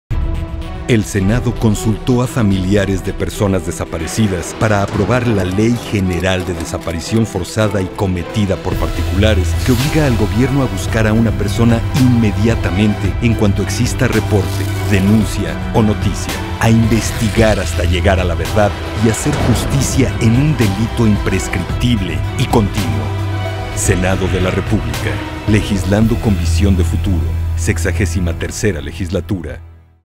Ley General de Desaparición Forzada obliga al gobierno a investigar hasta llegar a la verdad
El Senado consultó a familiares de personas desaparecidas para aprobar la Ley General de Desaparición Forzada y cometida por particulares que obliga al gobierno a buscar a una persona inmediatamente en cuanto exista reporte, denuncia o noticia; a investigar hasta llegar a la verdad y hacer justicia en un delito imprescriptible y continuo.